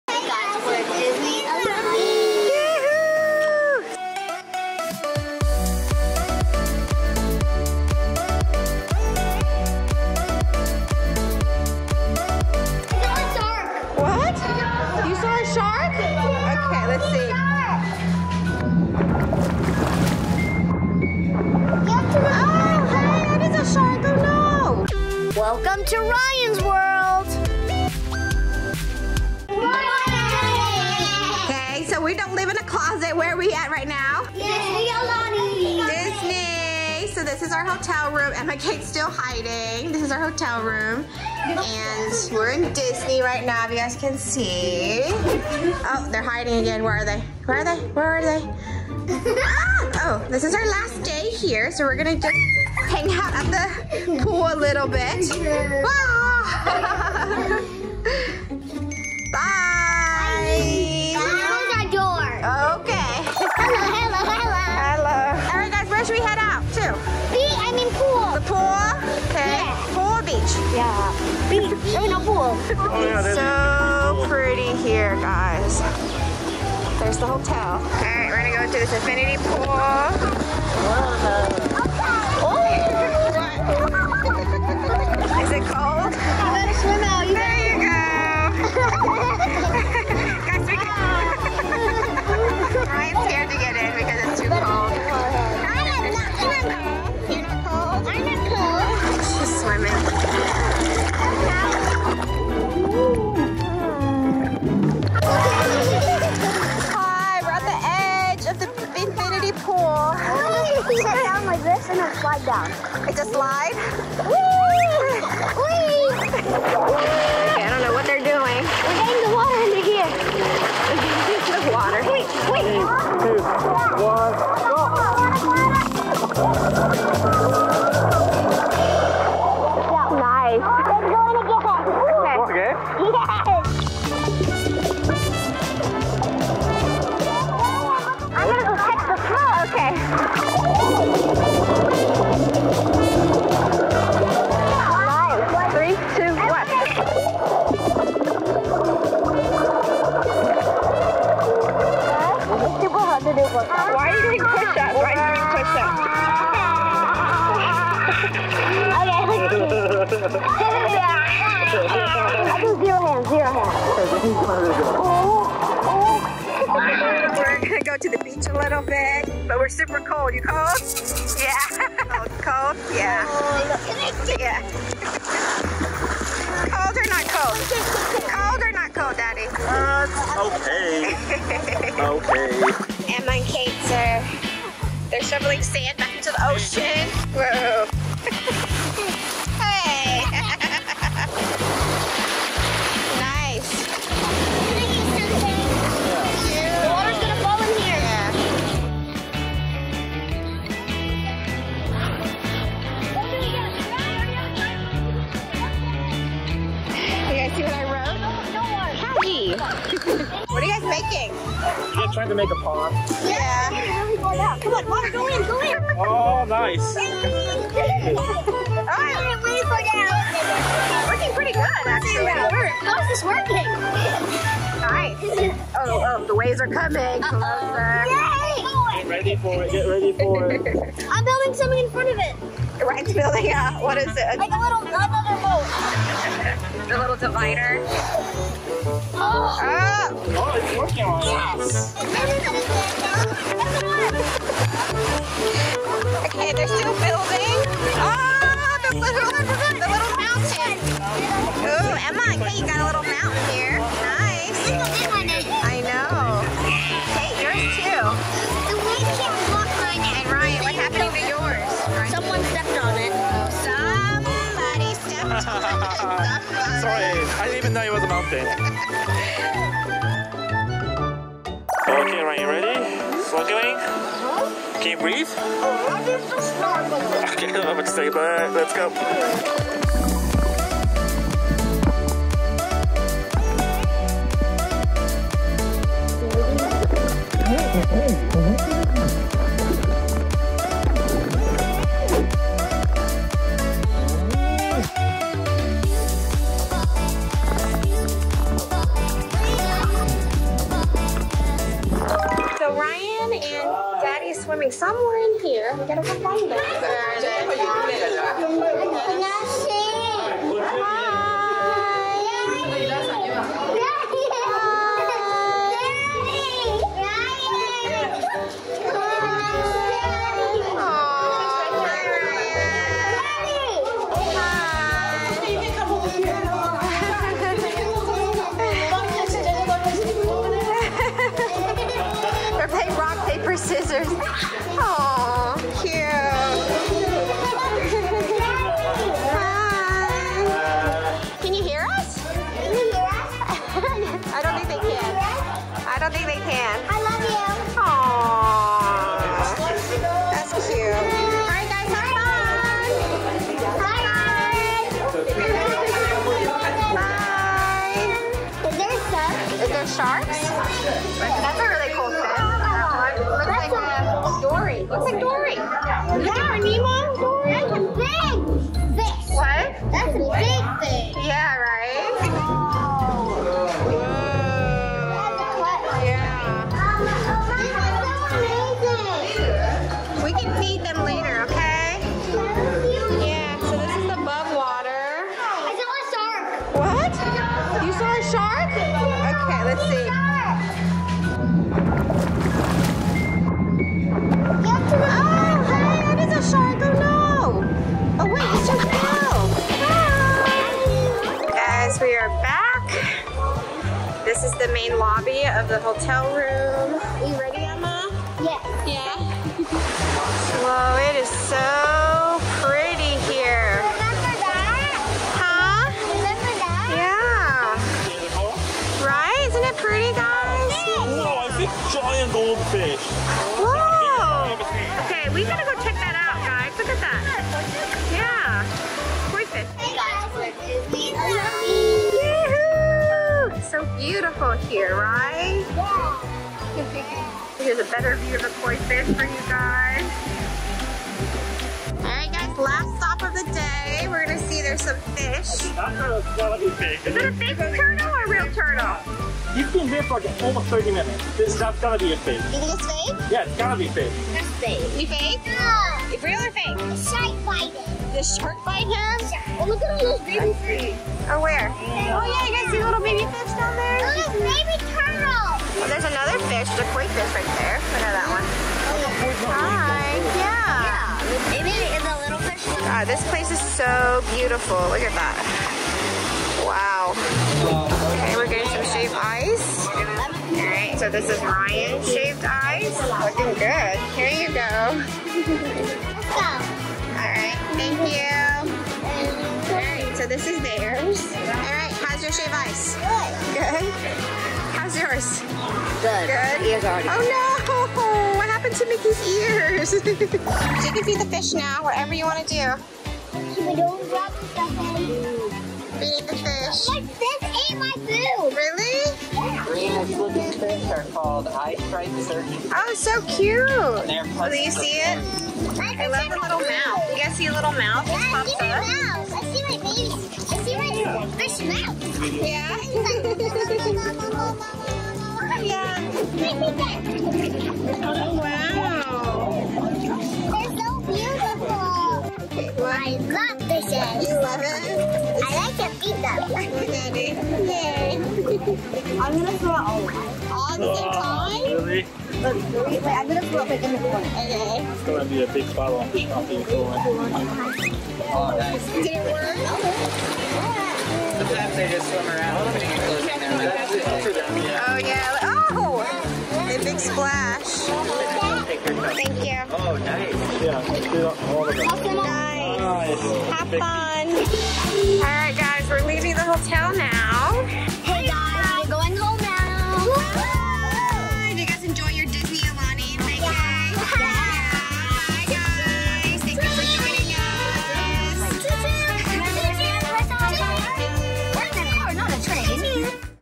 Ryan Swimming and Snorkeling at Disney Aulani In Hawaii!
Ryan Swimming and Snorkeling at Disney Aulani in Hawaii with Family! We had fun going to the Disney Resort and playing at the beach!